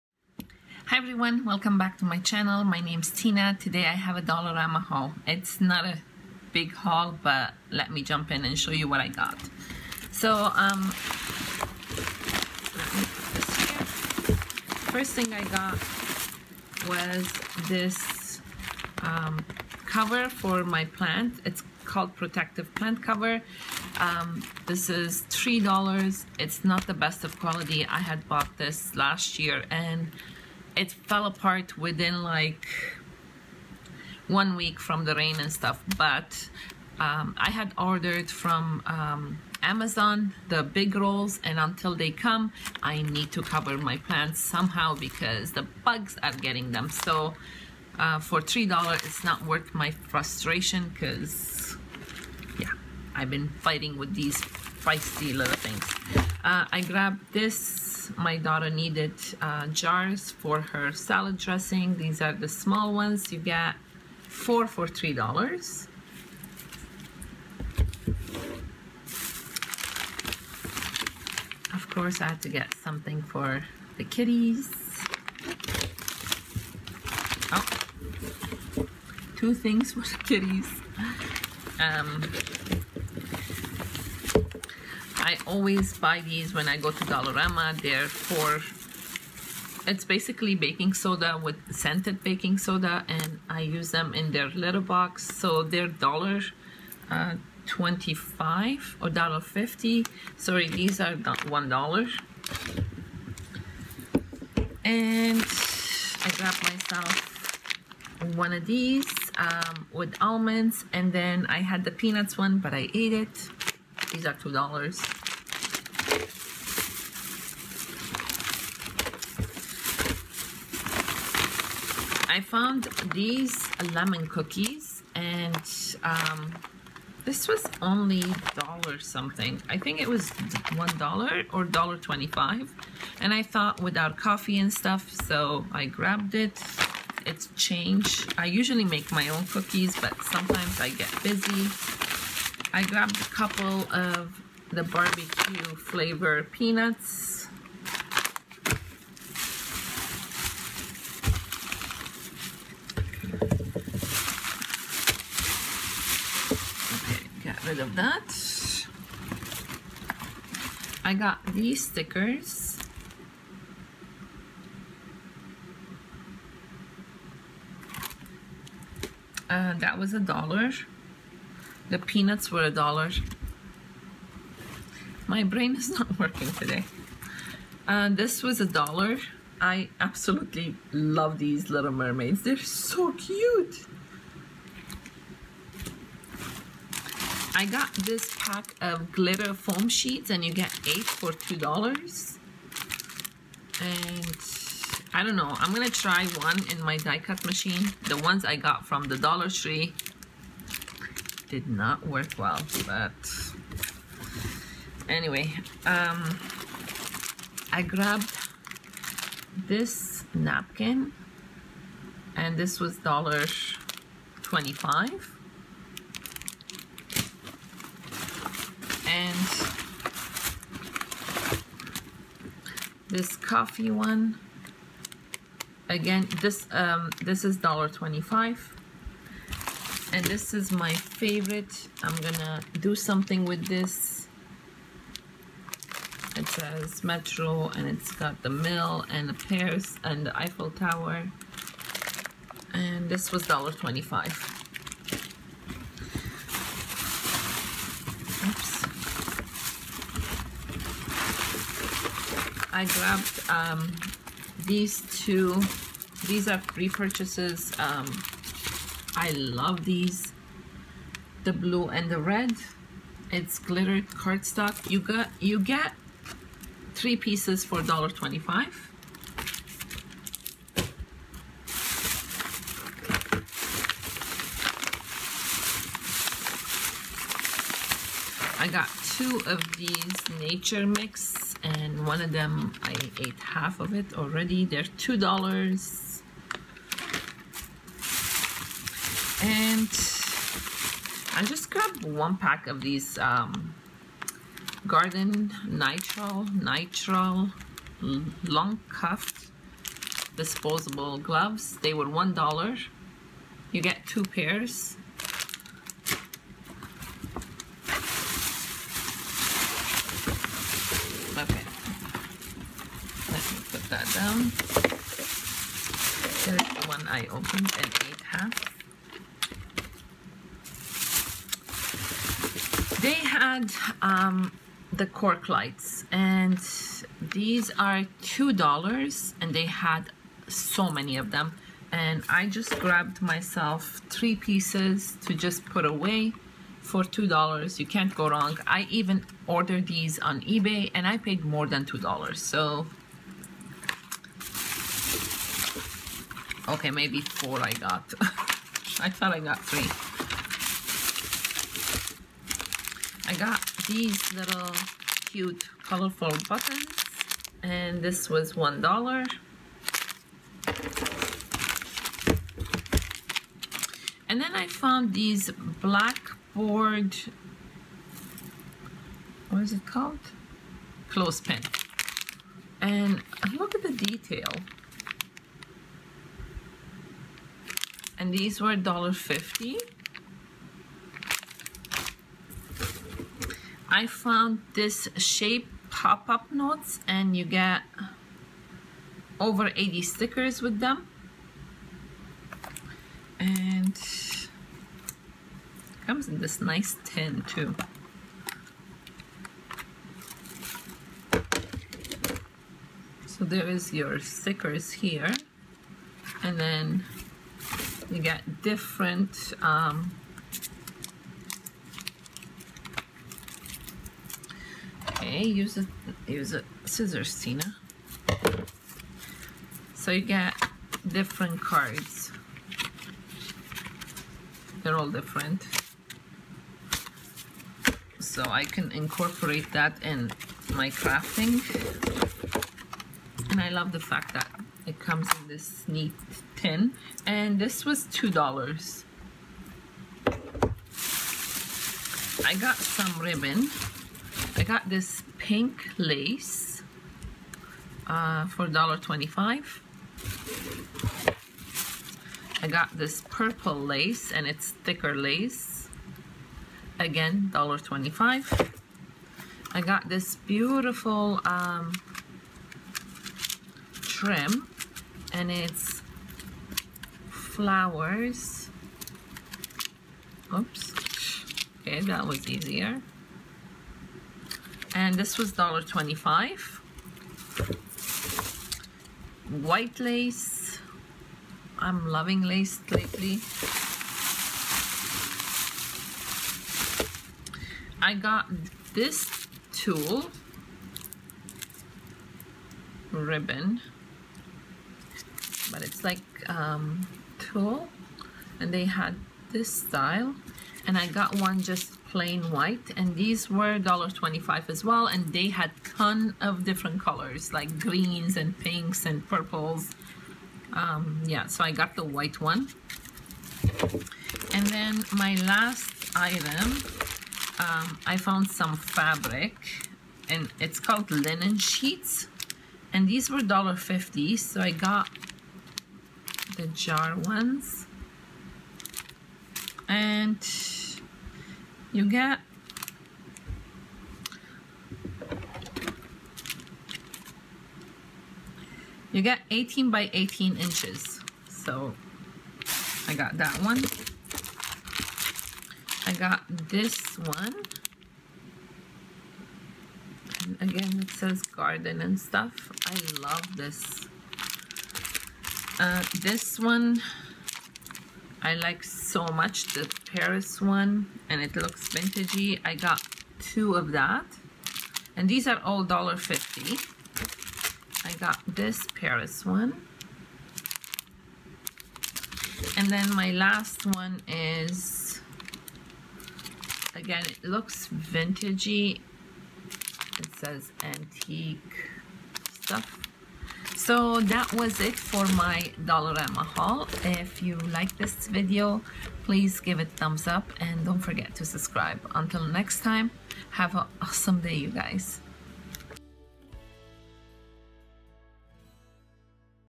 Dollarama Haul June 2018 😉
Hello everyone, today I have a Dollarama haul. Thank u so much for watching and have a wonderful weekend 💕